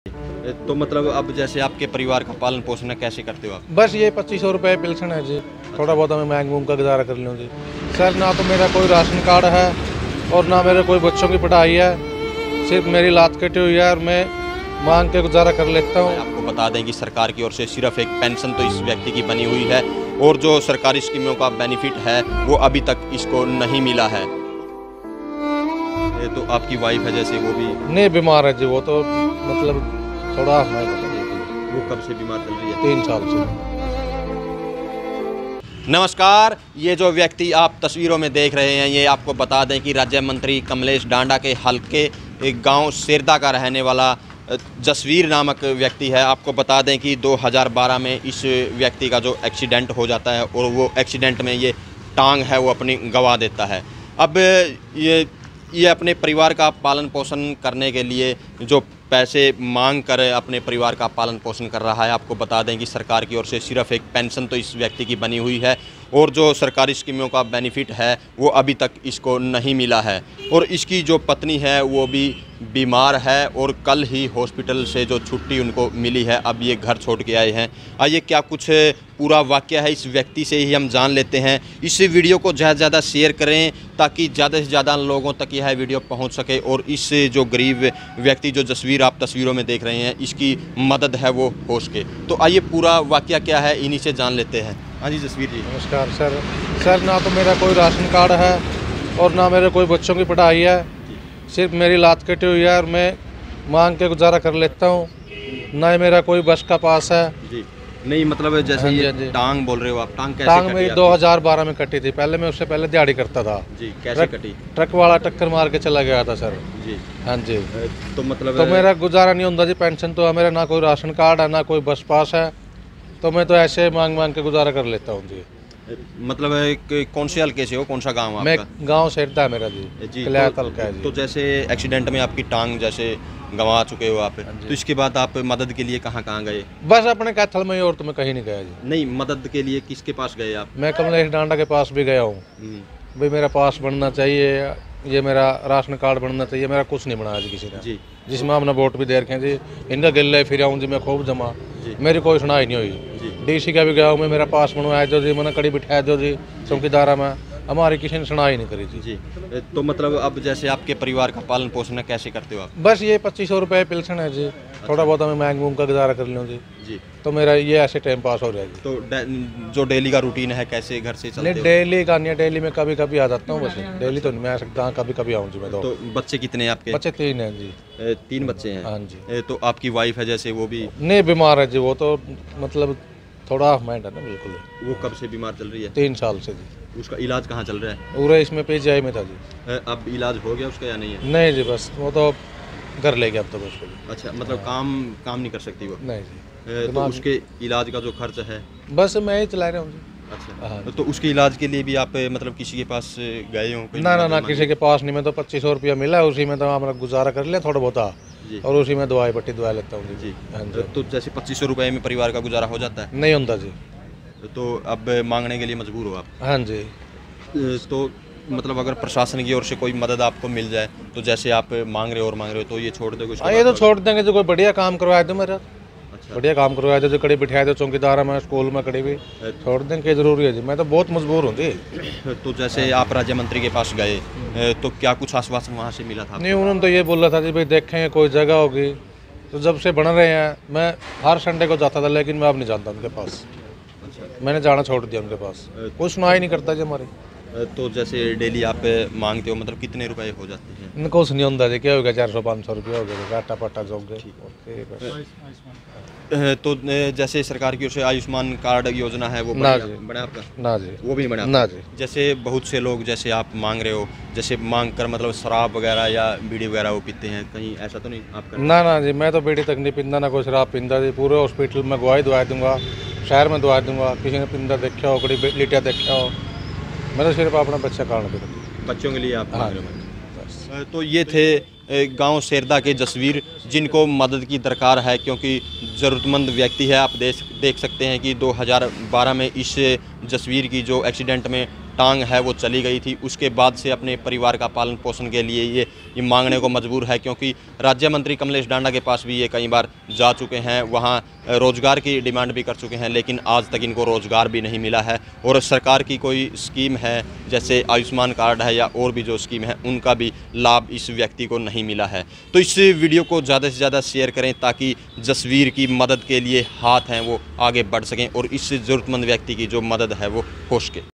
सेरधा के जसबीर को मद्द की दरकार...
-सडक़ हादसे में गंवा चुका है टांग, पत्नी भी बीमार
-राज्यमंत्री से कई बार लगा चुका है रोजगार की गुहार
-2500 से चलता है परिवार का गुजारा